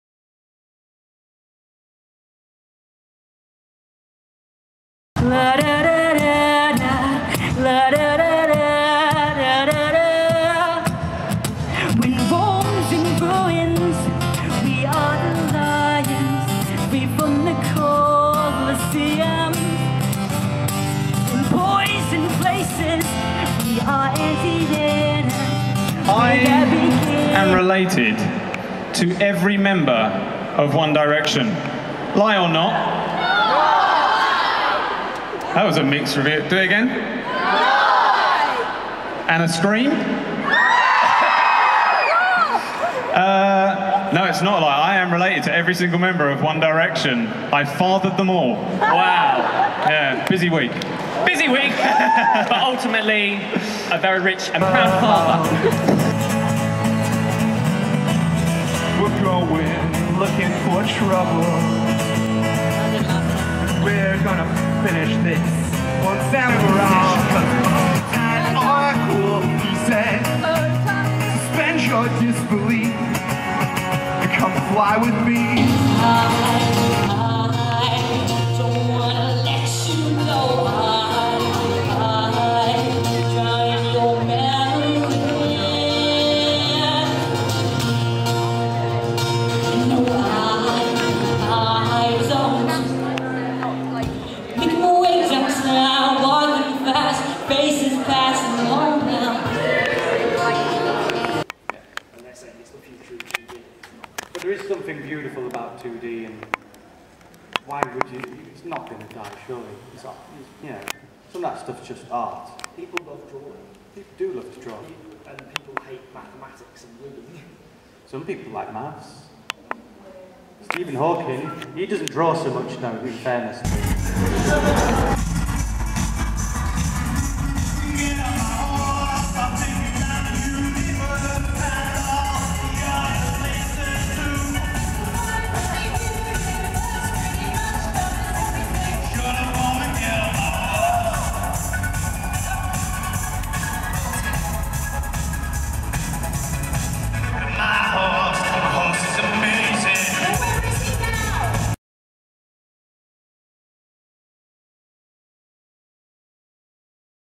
Summer in the City 2013: Day one
Here are just a few of my favourite moment from day one of Summer in the City.